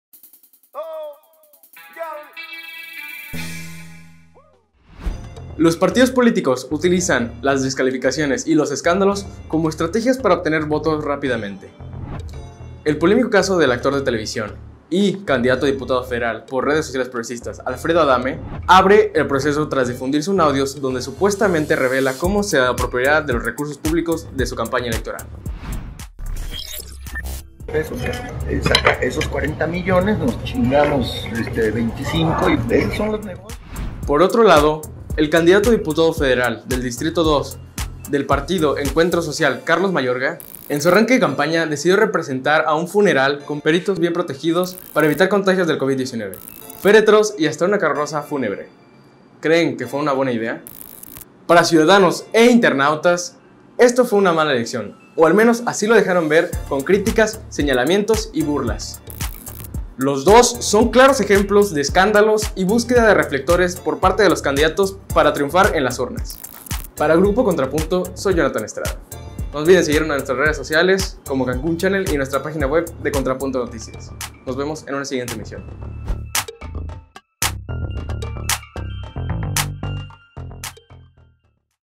ESCÁNDALOS Y DESCALIFICACIONES ANTE FALTA DE CAPACIDAD POLÍTICA
El actor Alfredo Adame y Carlos Mayorga, dos candidatos para diputación federal, inician campaña llena de polémica.